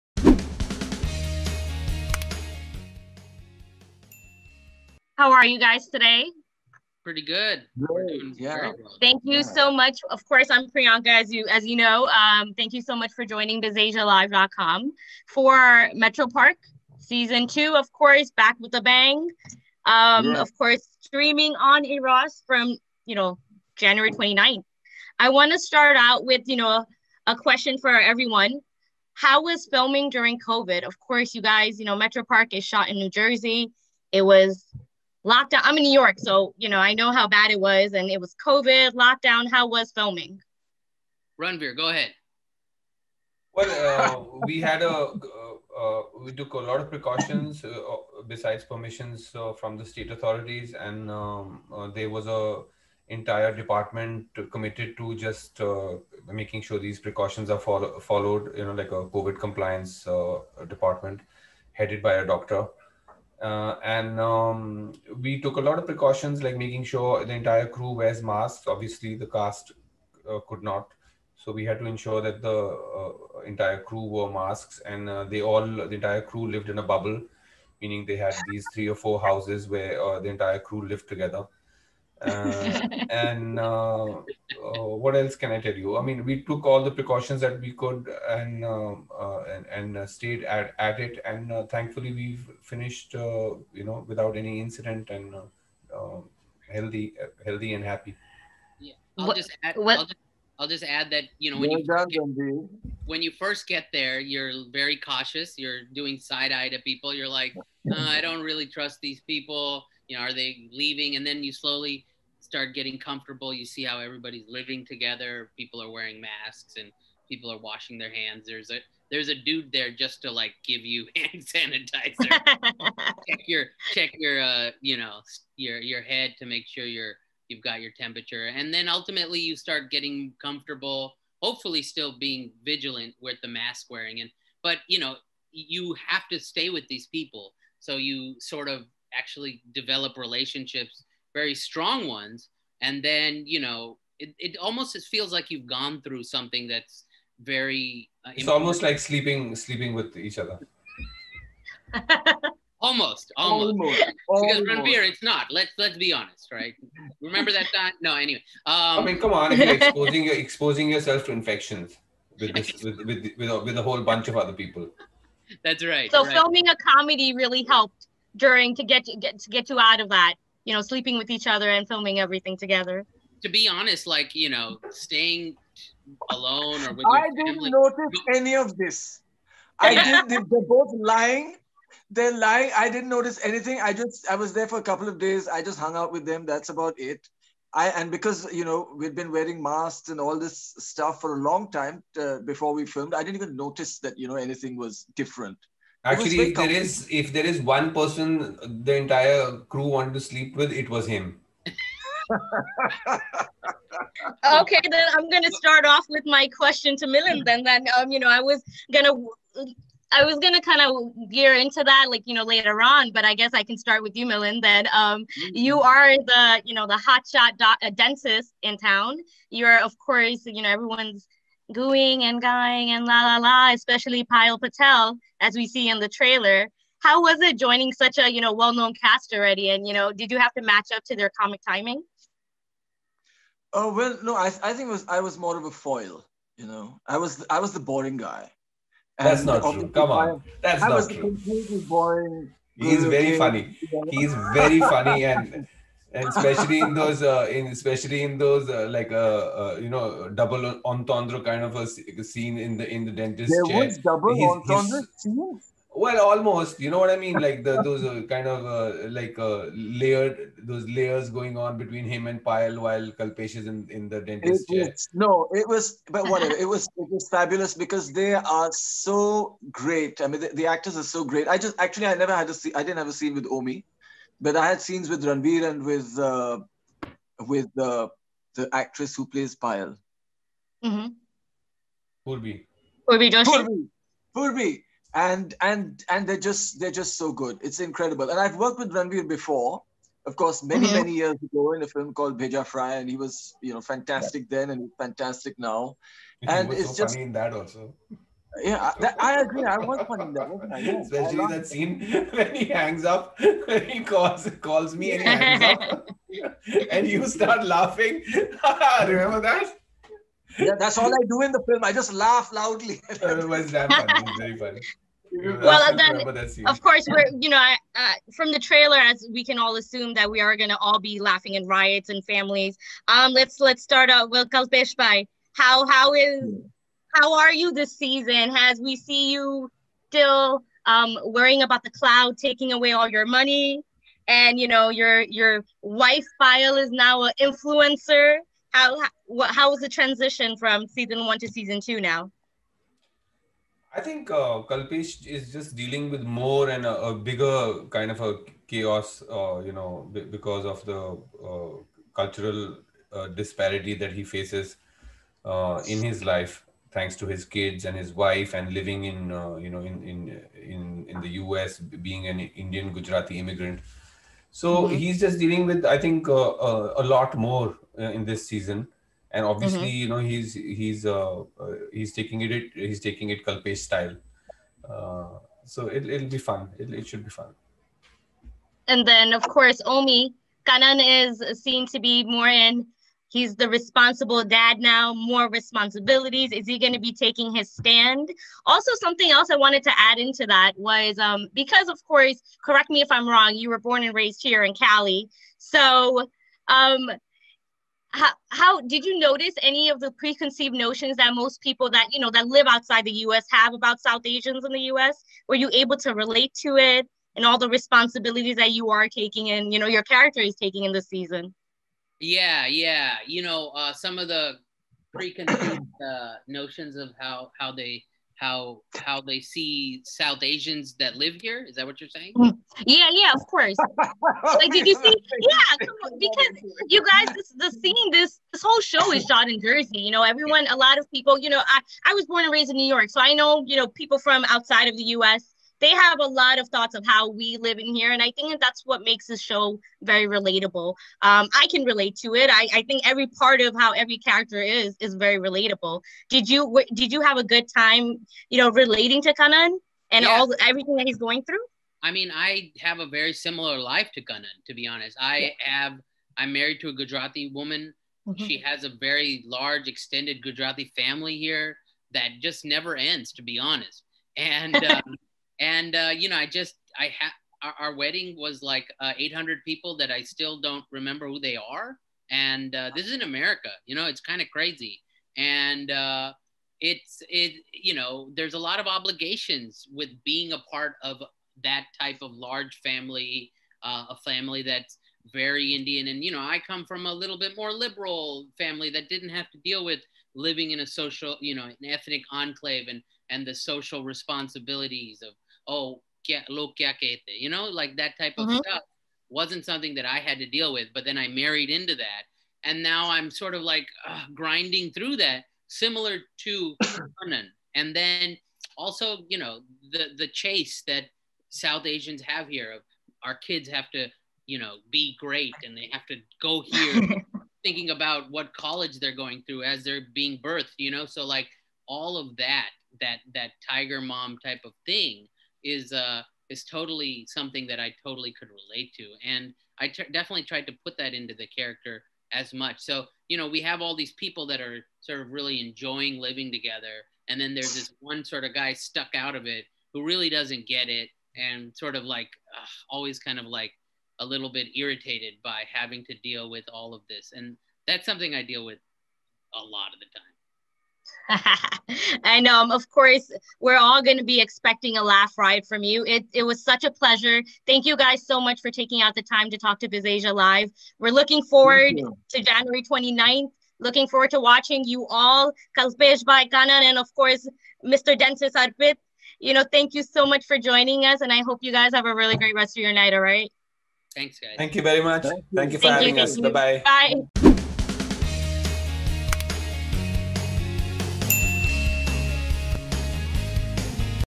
Priyanka Borua talks to Ranvir Shorey, Milind Soman & Omi Vaidya | BizAsia | Metro Park 2 | Eros Now
BizAsia's Priyanka Borua caught up with the leads of Eros Now's latest original 'Metro Park 2'  - Ranvir Shorey, Milind Soman and Omi Vaidya.

'Metro Park 2' is available now on Eros Now.